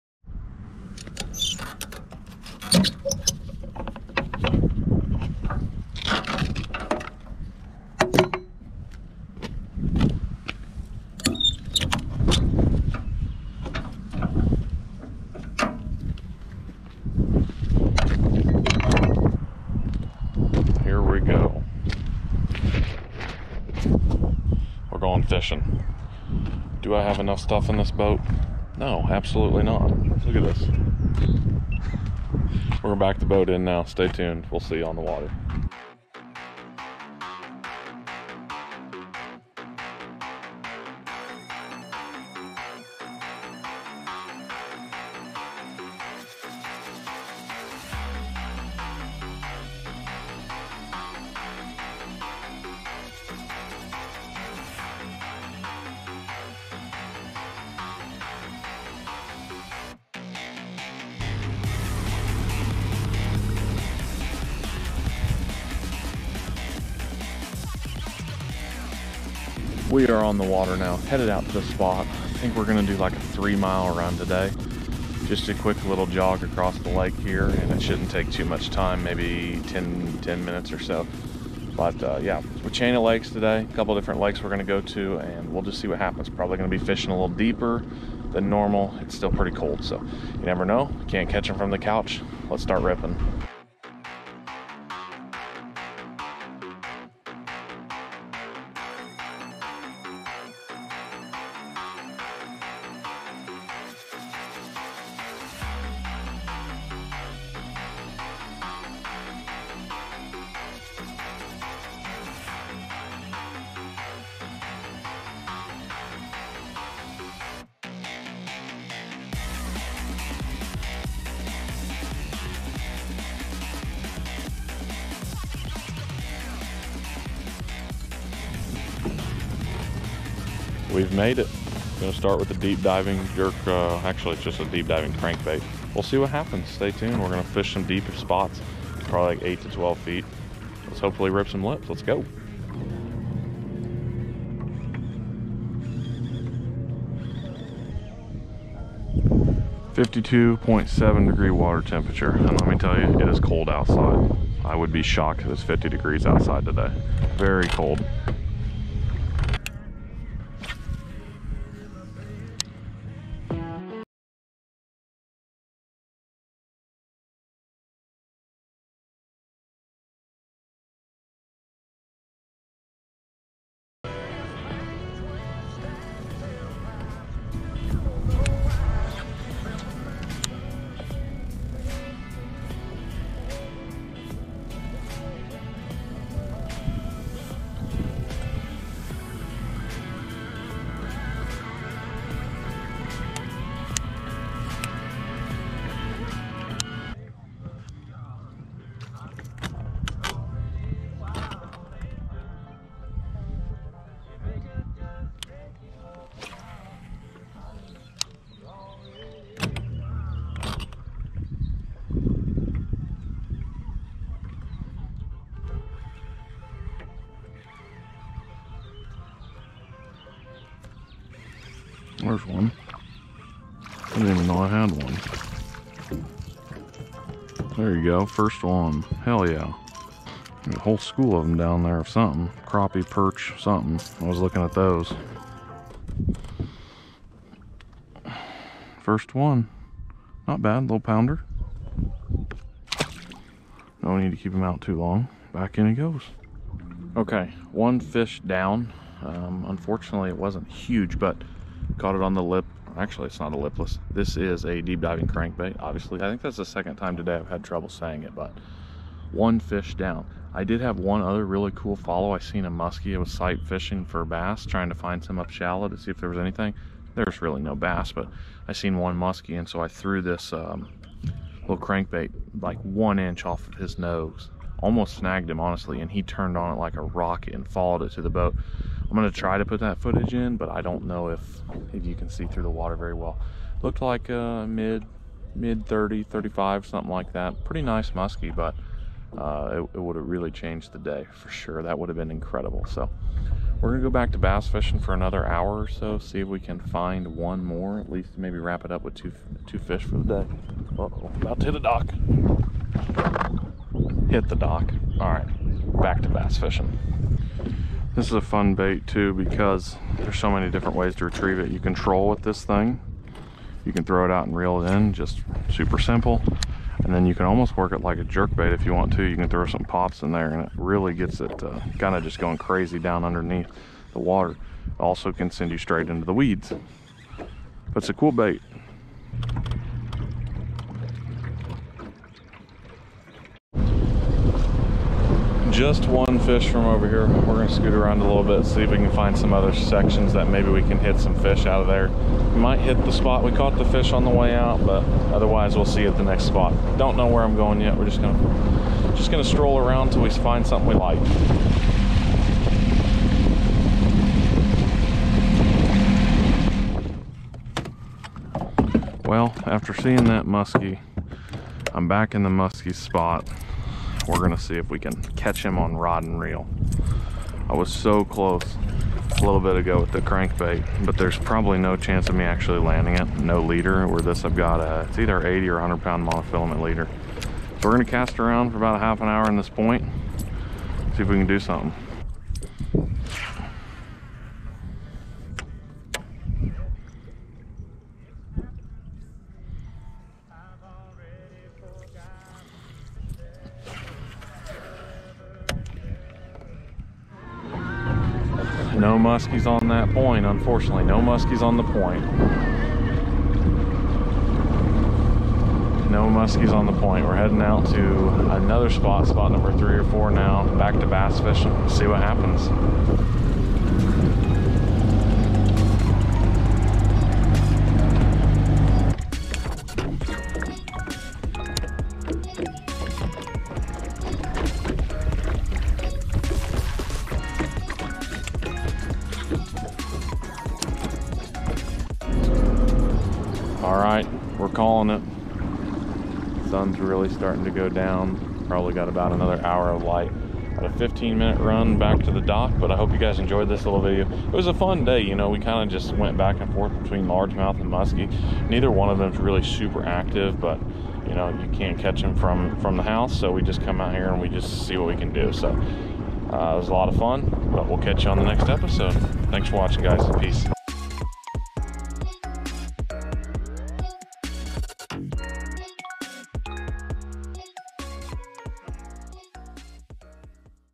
GIANT Musky while Bass Fishing!
Such a cool experience out on the water today hunting down more Green Fish!